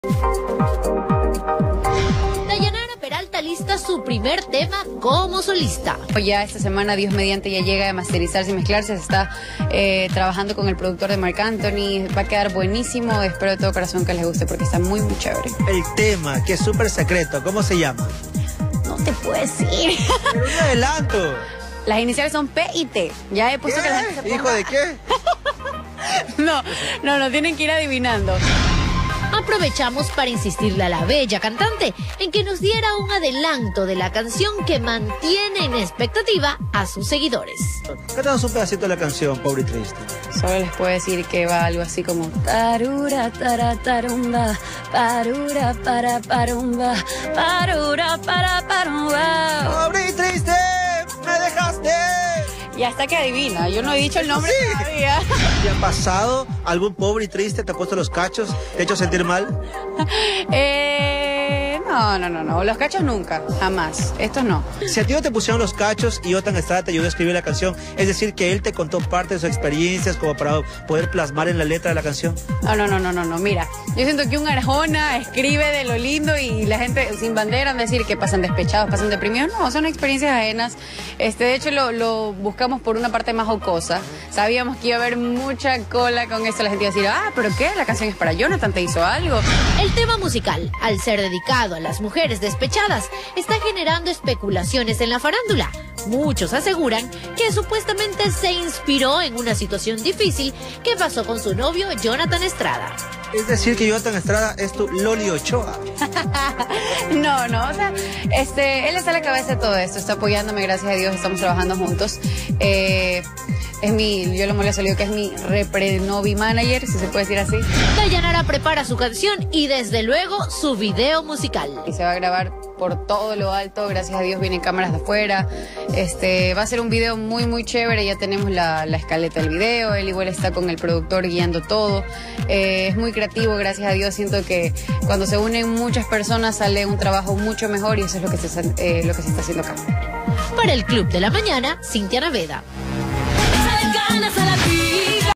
Dayanara Peralta prepara su primer tema
Dayanara Peralta alista su primer tema como solista y aprovechamos para insistirle que nos diera un adelanto.

Jonathan Estrada ayudó a escribir la canción